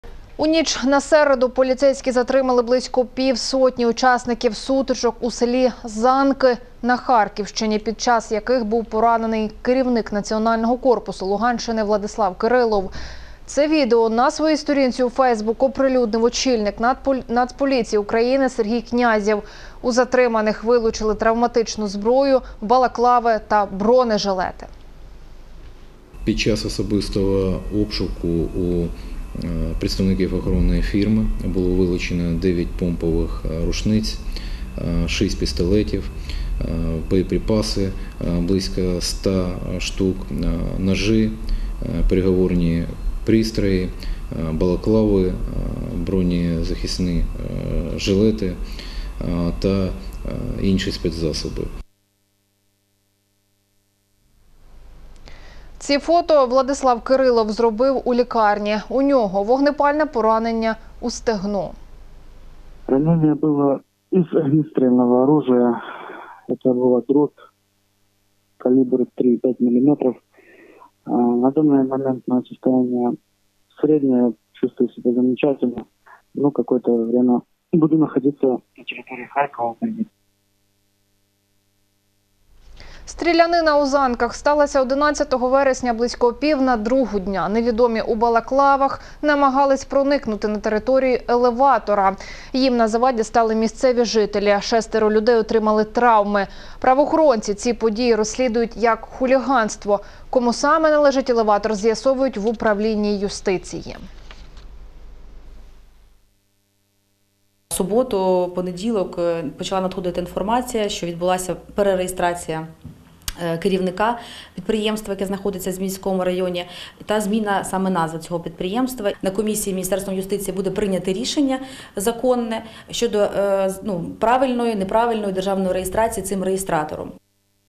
Поліція затримала учасників стрілянини на Харківщині
У ніч на середу поліція затримала близько півсотні учасників сутичок у селі Занки, на Харківщині, під час яких був поранений  керівник Національного корпусу Луганщини Владислав Кирилов. У затриманих вилучили травматичну зброю, балаклави та бронежилети.  Подробиці дивіться у сюжеті.